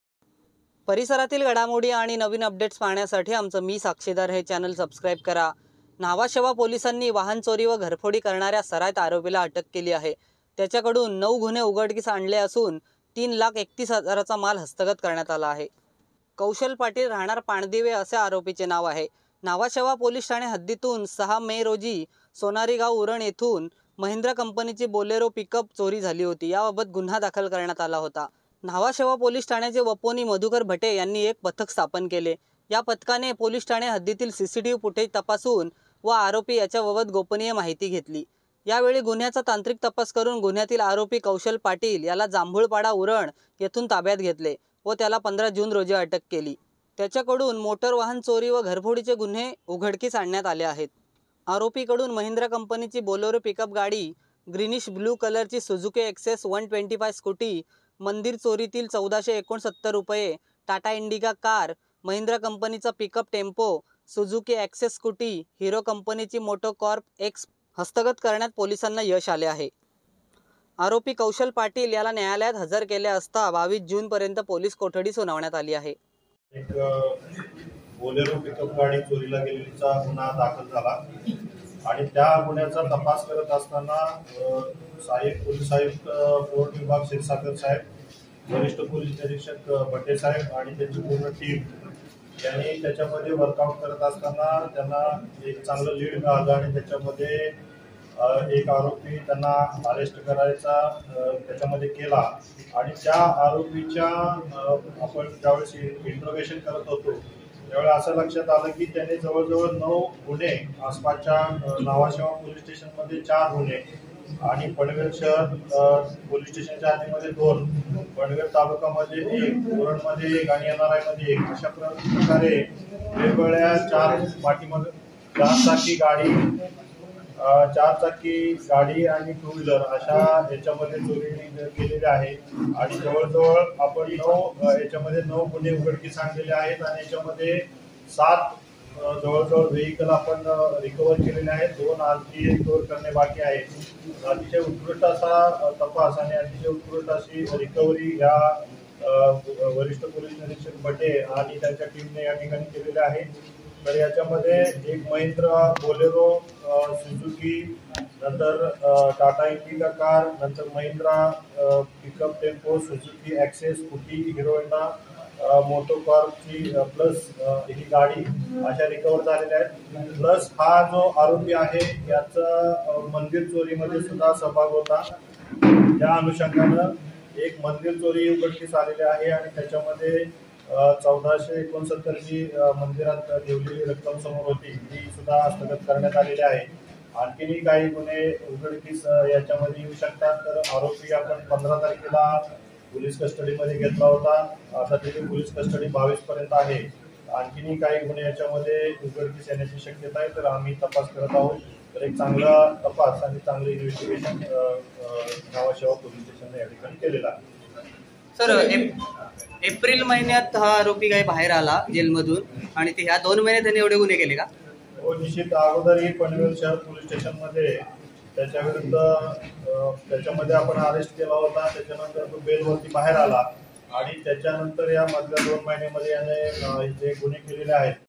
पाणदिव्याचा कौशल पाटील एप्रिलमध्ये आला जामिनावर,2 महिन्यात केले 9 गुन्हे, पोलिसांनी केली अटक
पाणदिव्याचा कौशल पाटील एप्रिलमध्ये आला जामिनावर,2 महिन्यात केले 9 गुन्हे, पोलिसांनी केली अटक#पाणदिवेpandive कौशलपाटील kaushal patil#न्हावाशेवाnhavasheva#